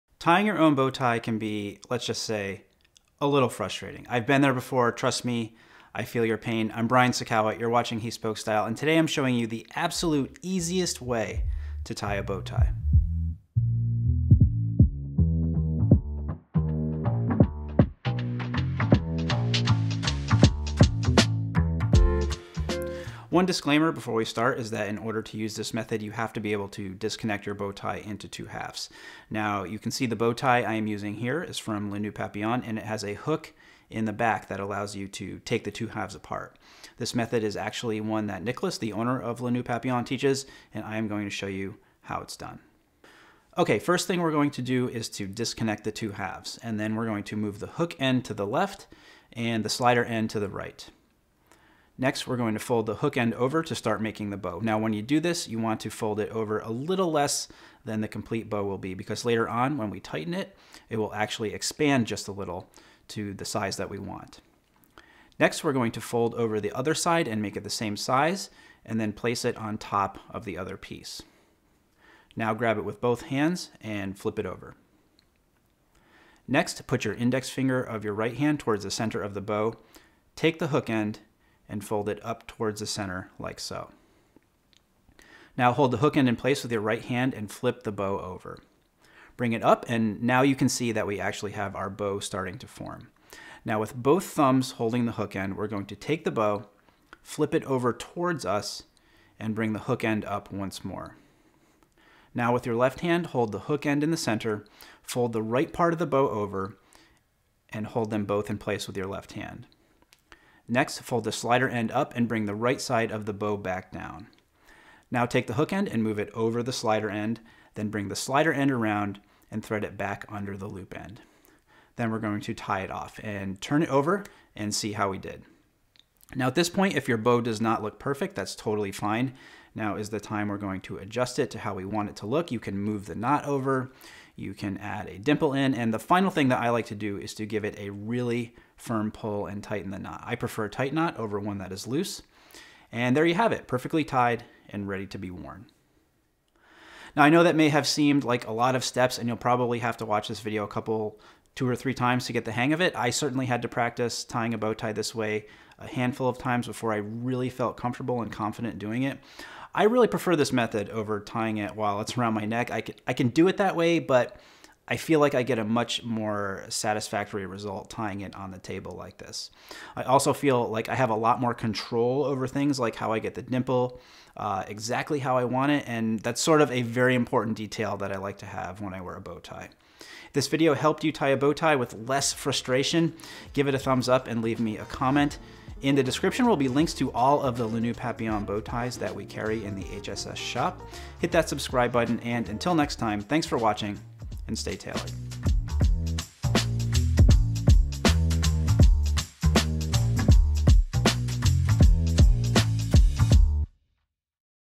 Easiest Way To Tie A Bow Tie | How To Tie A Bow Tie Step-By-Step
Sign up for the launch of our complete course, The Groom Guide

Tying your own bow tie can be a frustrating experience, especially if you are a beginner. However, wearing a self-tie bow tie is the mark of a true gentleman, so it behooves you to finally dig in and learn how to do it! In this video, I'm showing you, step-by-step, an easy way to tie a bow tie.

Full disclosure: the method taught in this video does not work with all bow ties. You must be able to disconnect the two halves of the bow tie in order to tie a bow tie this way.

The key to this method is not actually tying the bow tie while it is around your neck. Instead, you tie it on a table (or any flat surface).

I much prefer tying my bow ties this way and here are the reasons why:

1. Less frustrating
2. More control over details like dimples
3. Perfect every time

It takes just a bit of practice, but if you learn how to do it this way, you'll be tying your bow ties faster and better than ever.
LE NOEUD PAPILLON BOW TIES

All Le Noeud Papillon bow ties in the HSS Shop are available in either satin or grosgrain.